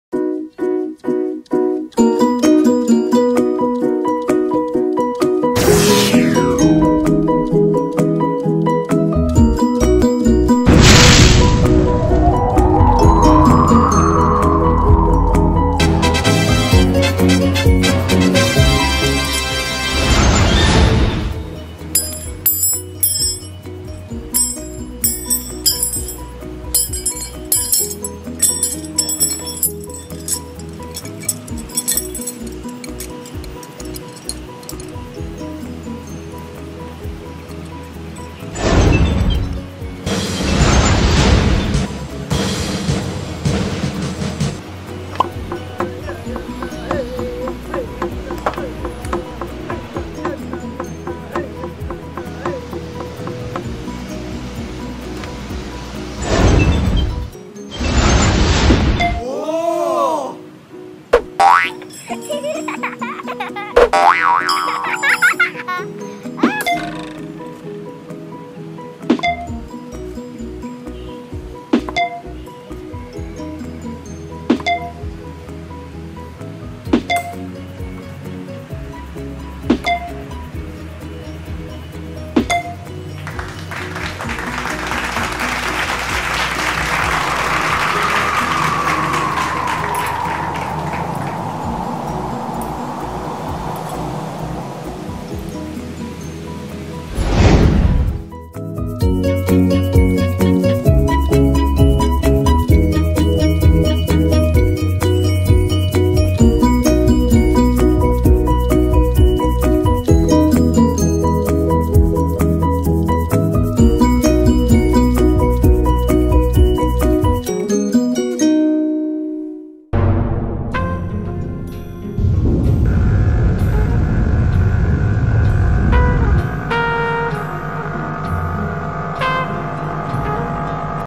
No Spin Shuriken Exercise | JKT1 | Thor ASJP | D'LEMPAR PISAU INDONESIA
Throwing practice at shuriken nospin way at taman Langsat Jakarta. No matter how busy i am, tried to have fun my self, throwing. I can spell c-r-a-z-y is even more better than l-a-z-i.. Oops!  l-e-z-y! Err.. L-a-z-y!